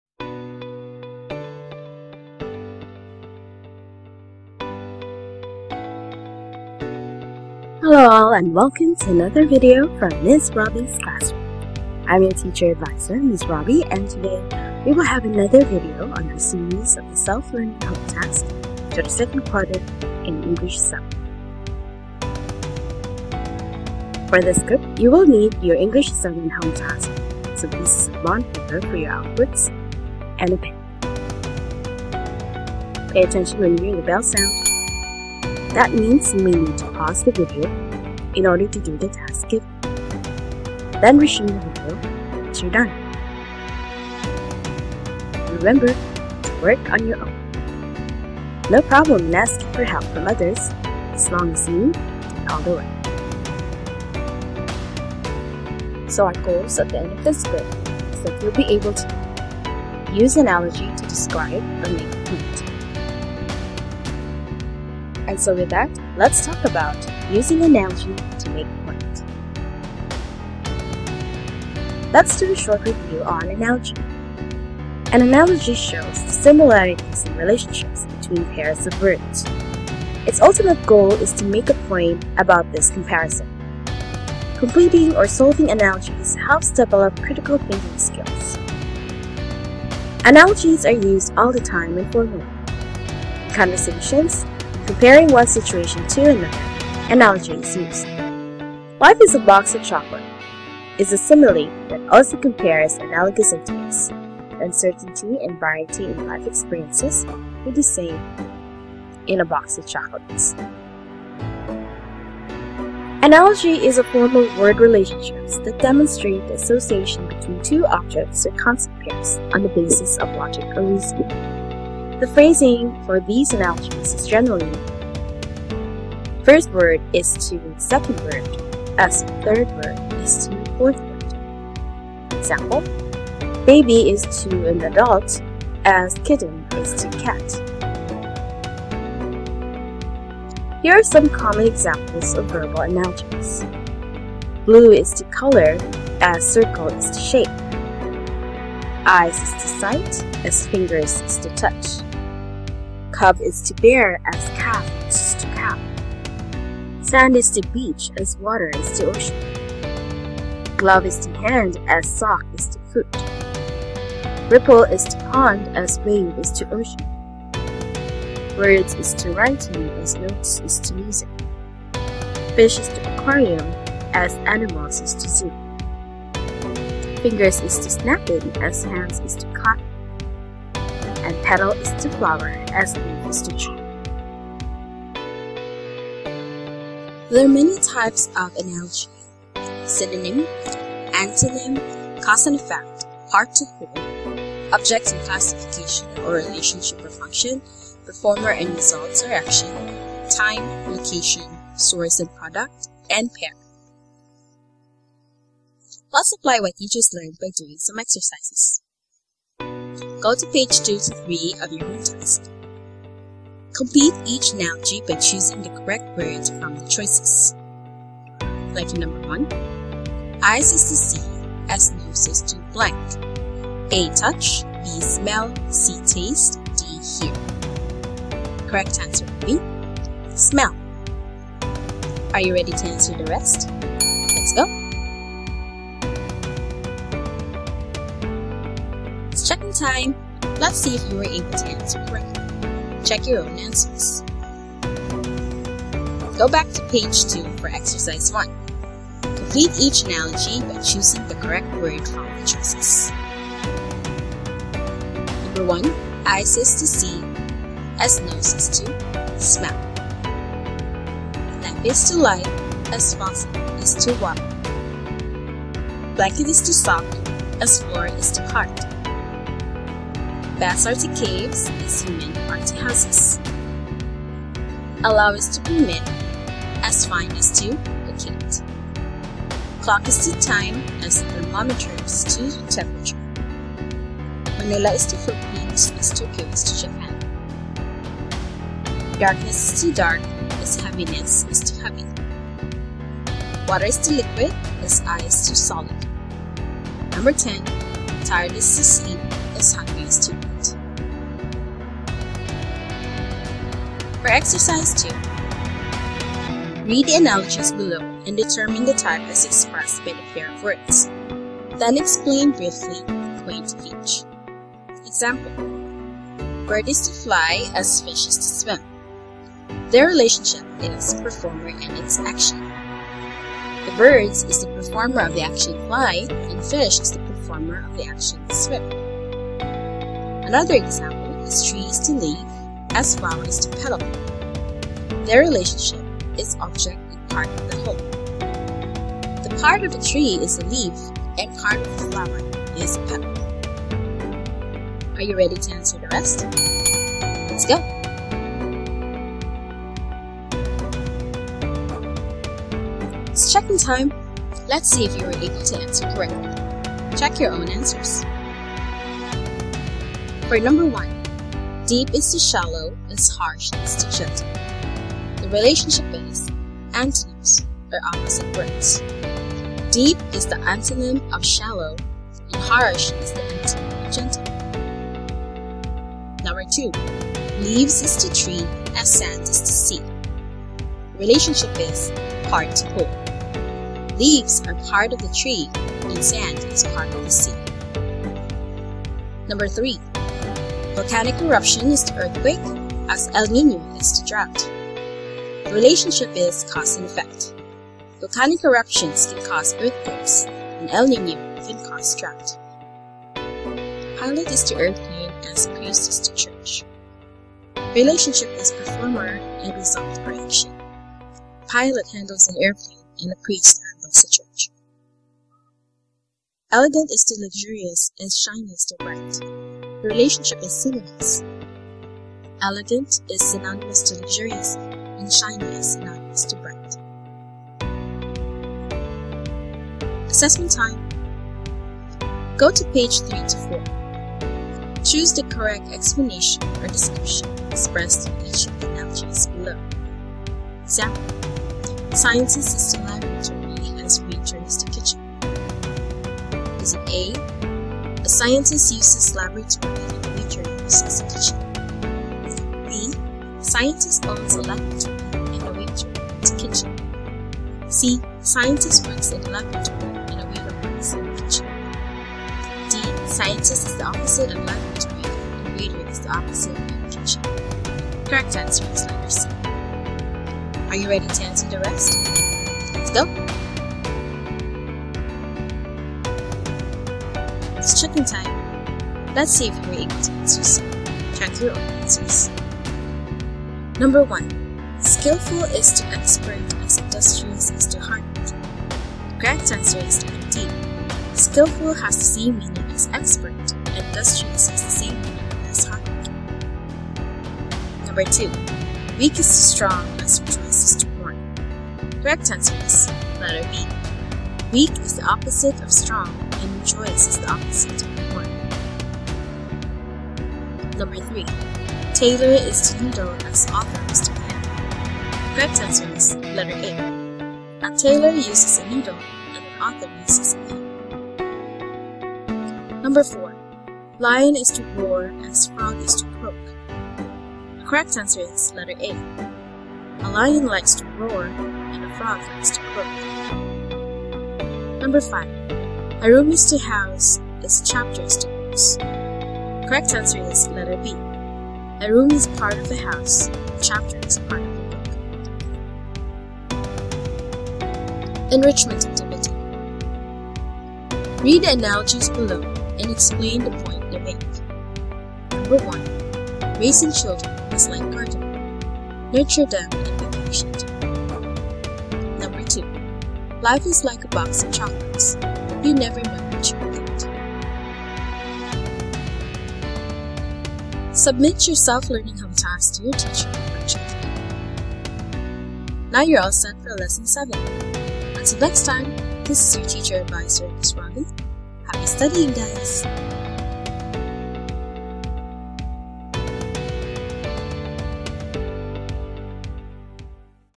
ENGLISH 7 Q2 SLHT WEEK 6: USING ANALOGY TO MAKE A POINT
A video guide on the Self-Learning Home Task (SLHT) for ENGLISH 7 by Deped Cebu Province written by CATHERINE R. OFQUERIA.